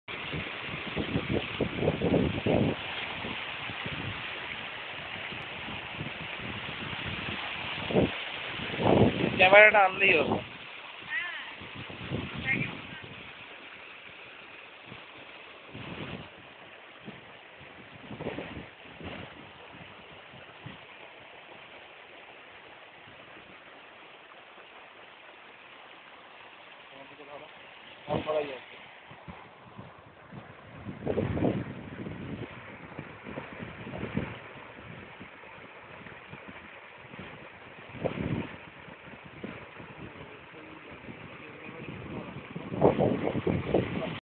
Outing in Balarampur
The day I got on a little outing with my wife. It was a sunday afternoon. After a heavy and long splash the weather was pretty romantic. The beautiful landscape that was running along with us was really breathtaking. We stopped beside a little brook to enjoy it freshness and be invigorated by its spirit.